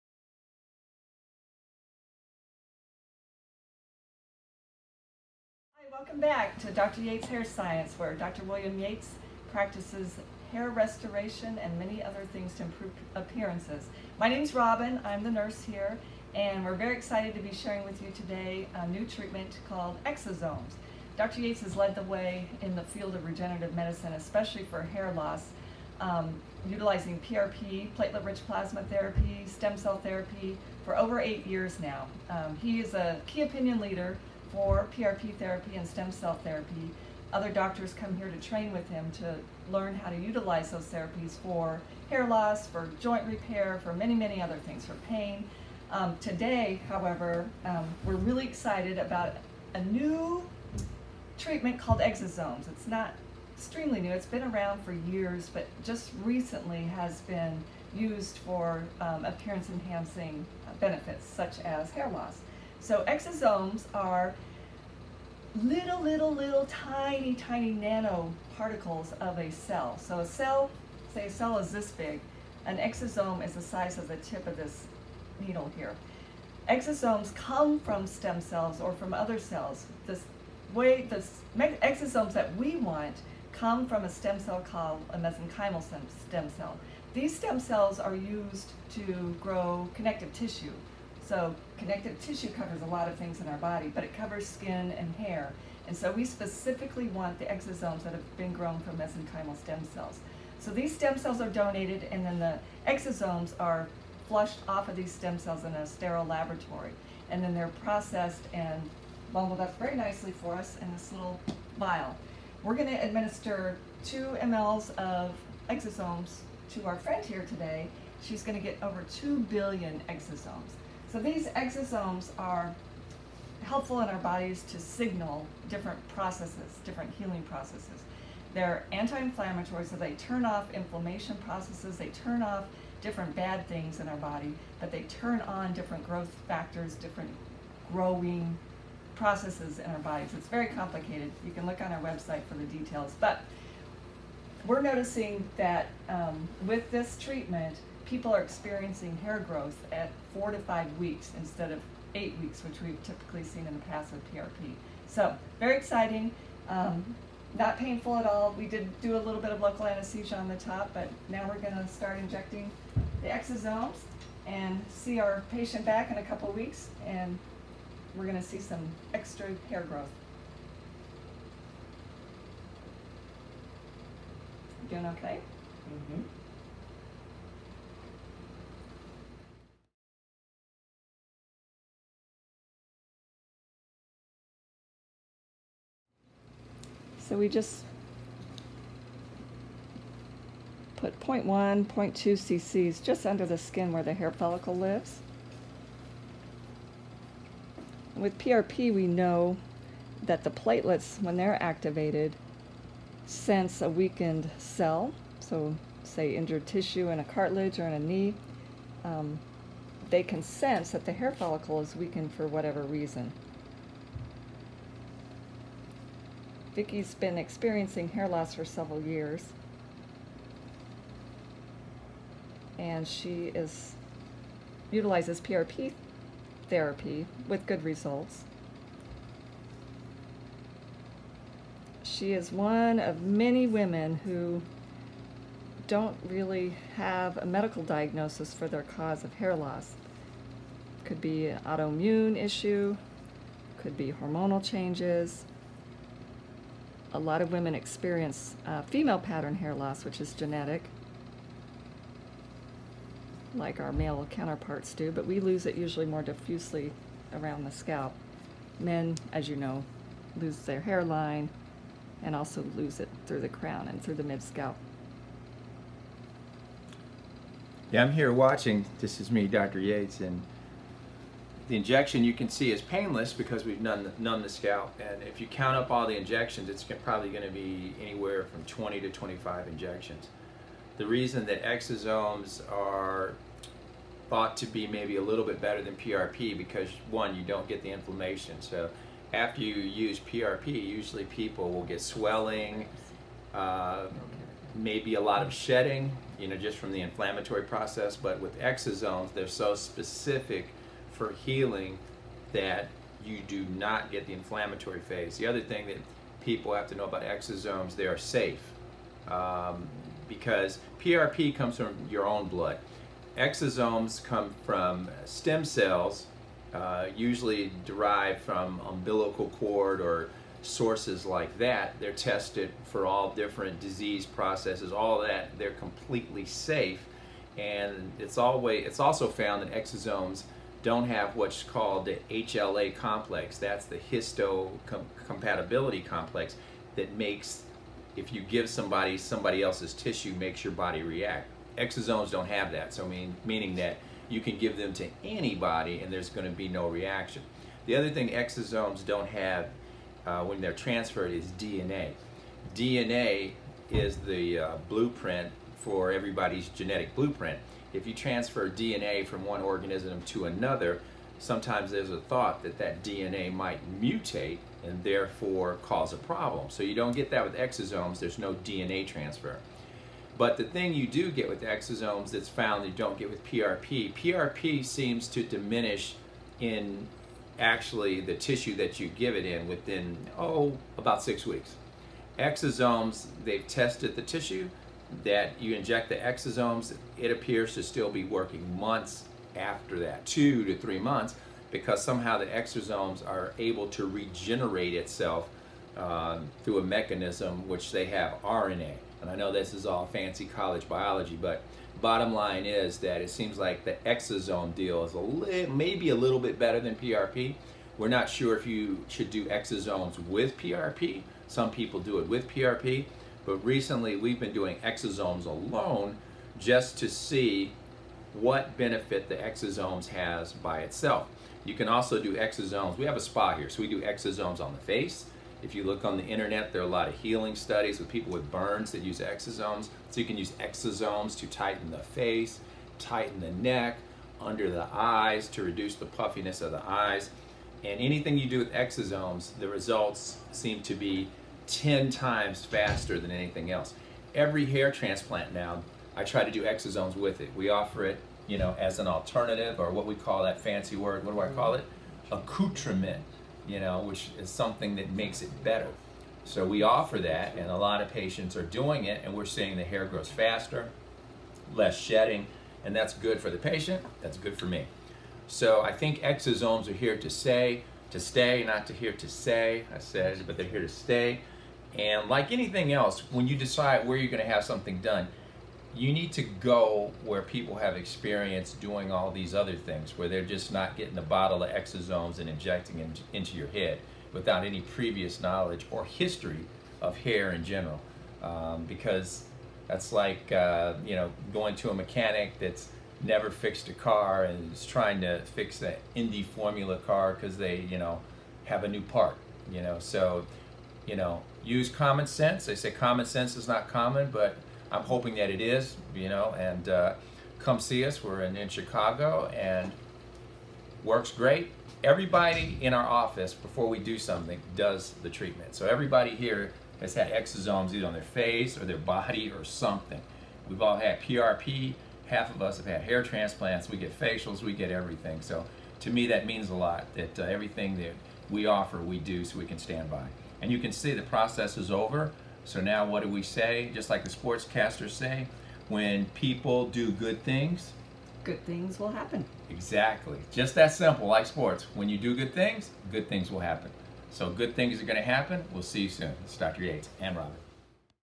EXOSOMES: The revolutionary restorative treatment!
New to the Hair restoration industry, Dr Yates in a pioneer in the use of Exosomes. This nanoparticles of proteins are extracted from Stem Cells and have super healing properties.
Results show faster than other restoration treatments.